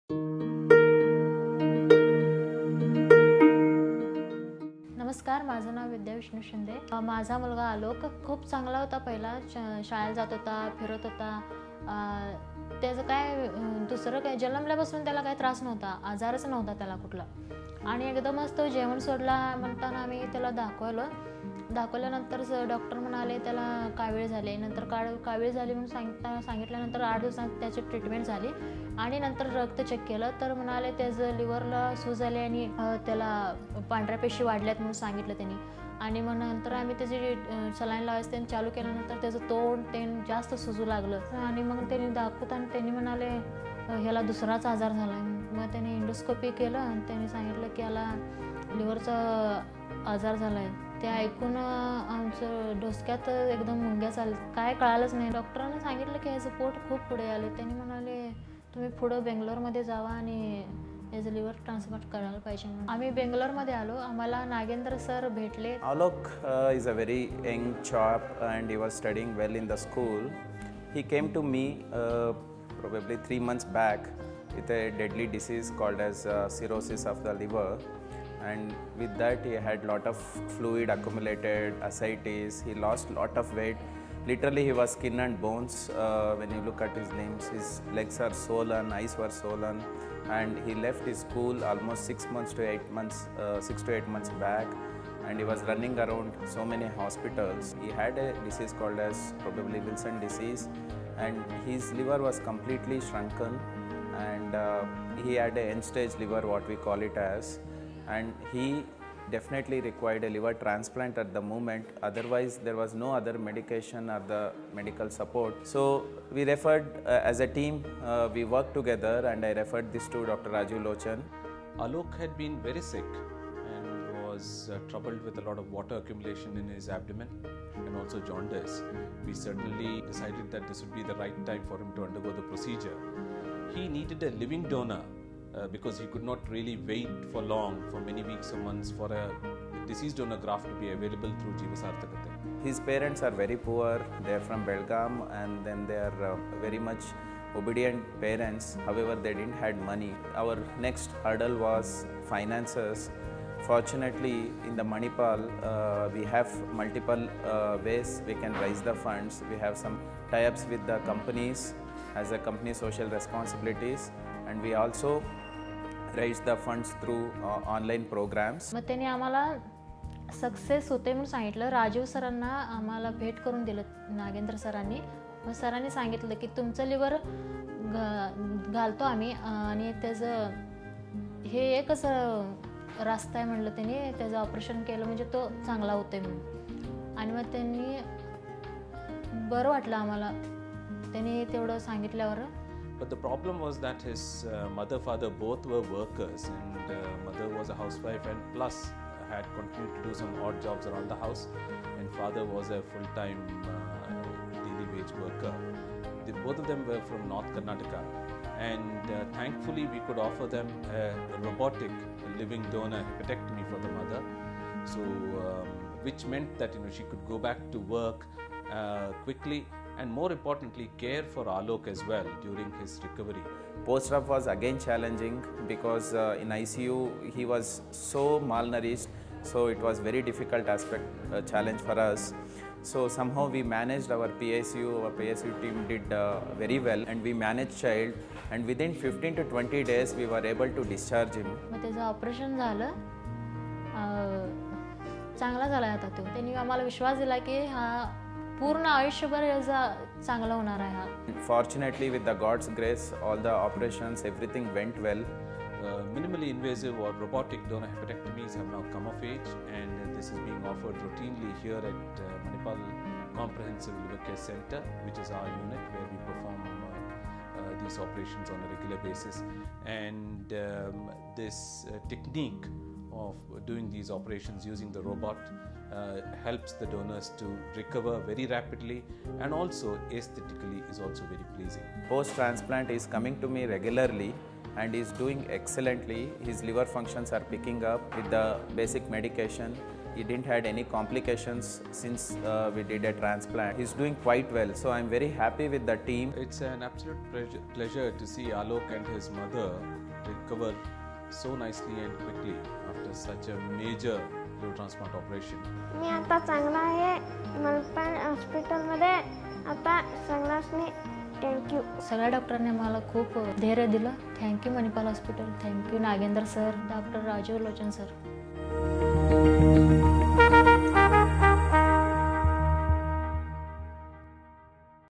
Testimonial |Dr. Nagendra Kumar V. R. and Dr. Rajiv Lochan | Liver Transplant - Manipal Hospitals
Dr. Nagendra Kumar V. R., Consultant - Pediatric Gastroenterology, Hepatology and Nutrition, and Dr. Rajiv Lochan, Lead Consultant - HPB and Liver Transplantation Surgery, Manipal Hospital Old Airport Road, recently treated a young patient from Belgaum named Alok. He was diagnosed with liver cirrhosis and required a transplant. His complex case was addressed with a multispeciality approach, ensuring a smooth procedure and quick recovery. Watch this video to know more.